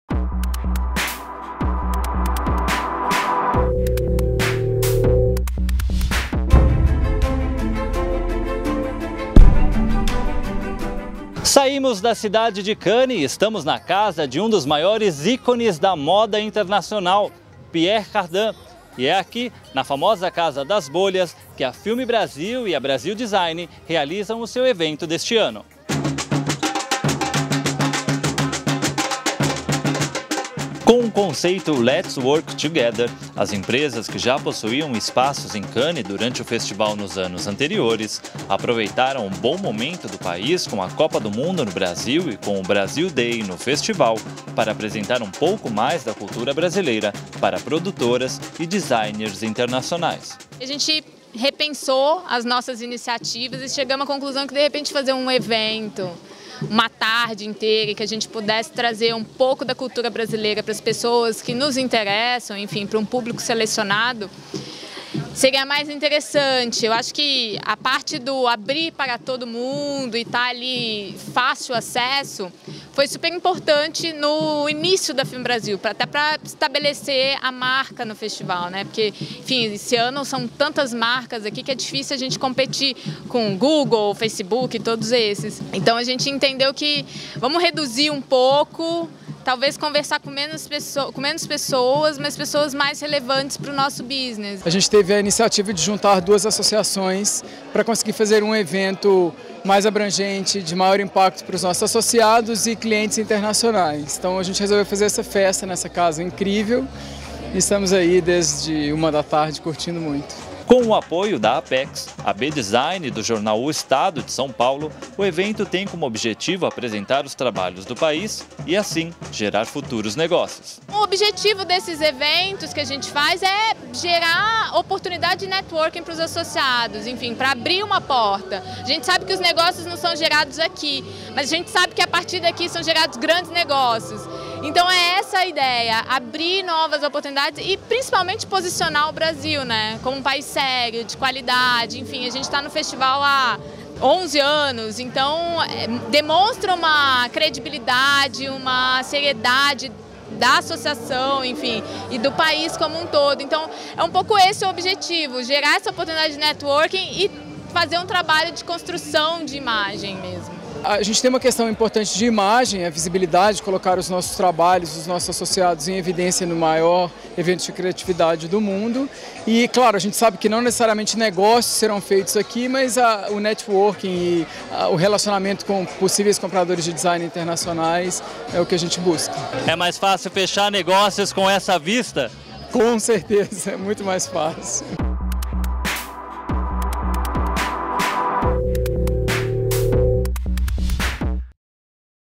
Festa com jeito brasileiro para gerar negócios
Com o conceito 'Let's work together', FilmBrazil e Brasil Design promovem evento na casa do estilista Pierre Cardin durante o Festival de Cannes com o objetivo de gerar futuros negócios.